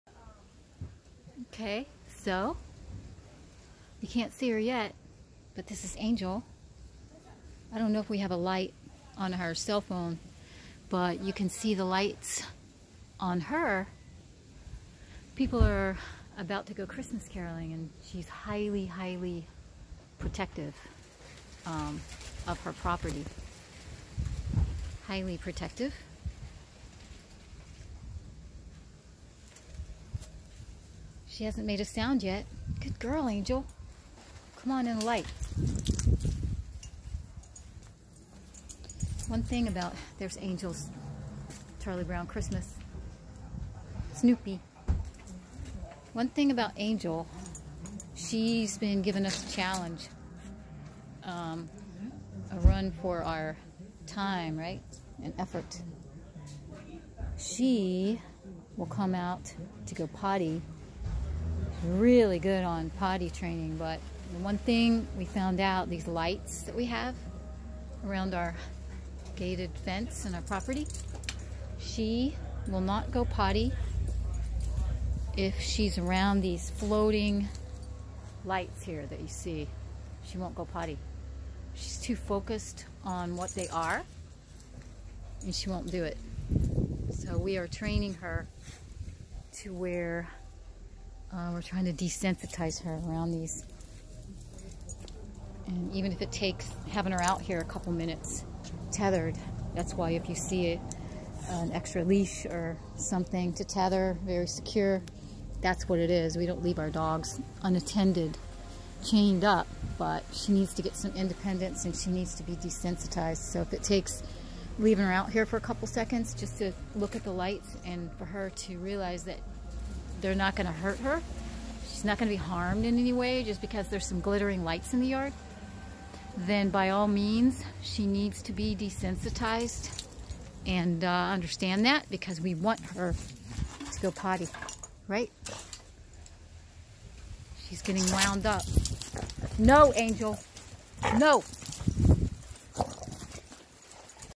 Sioux City Pit Angel works through Fear
Potty training through fear issues with sounds, wind in trees, and floating Christmas lights.  Temper tantrums dont get you what you want in our HUGHeartsFoundation rehab.